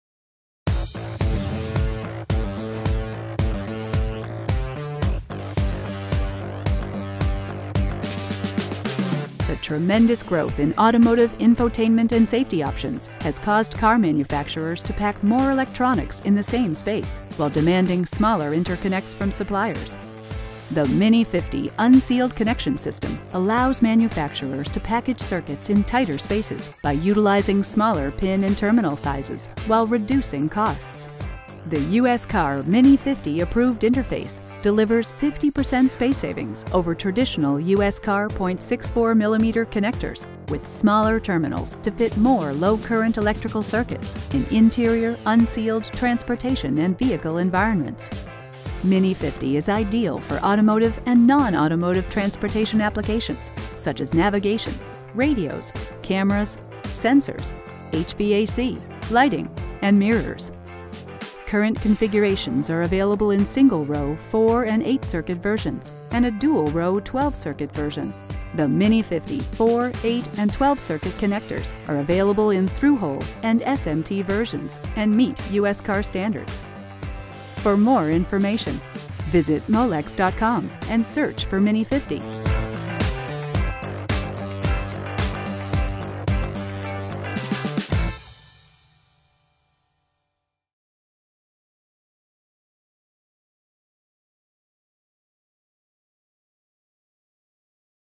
Mini50 Unsealed Connection System | Molex
The Mini50™ Unsealed Connection Systems allows manufacturers to package circuits in tighter spaces by utilizing smaller pin and terminal sizes while reducing costs.

The tremendous growth in automotive infotainment and safety options has caused the car manufacturers to demand smaller interconnects from suppliers. Manufacturers need to fit more electronics in the same space.

The USCAR Mini50™ approved interface, delivers 50% space savings over traditional USCAR 0.64mm connectors, with smaller terminals to fit more low-current electrical circuits in interior, unsealed, transportation-vehicle environments.

Mini50™ is ideal for Automotive and non automotive transportation applications such as Navigation, Radios, Cameras, Sensors, HVAC, lighting , and mirrors.

Current configurations are available in single-row, 4- and 8-circuit versions and a dual-row, 12-circuit version. The Mini50™ 4, 8 and 12 circuit connectors are available in through-hole and SMT versions and meet USCAR standards.

Visit Molex.com and search for Mini50™ for more information.